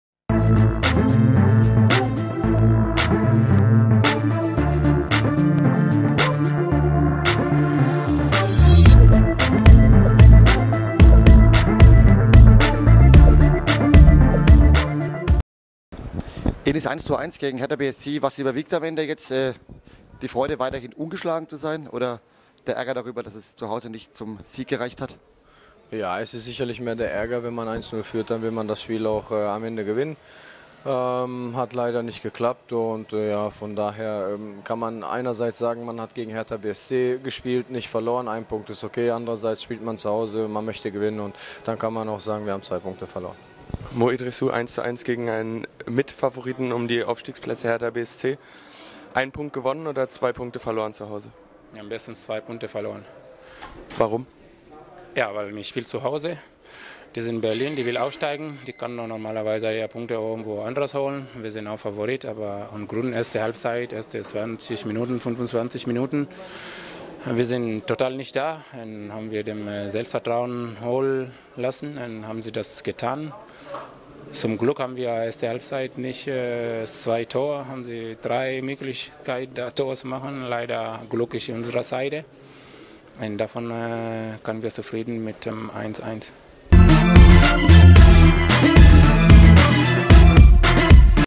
Stimmen nach dem Spiel gegen Hertha BSC
Enis Alushi und Mohamadou Idrissou stellen sich den Fragen des FCK- TV´s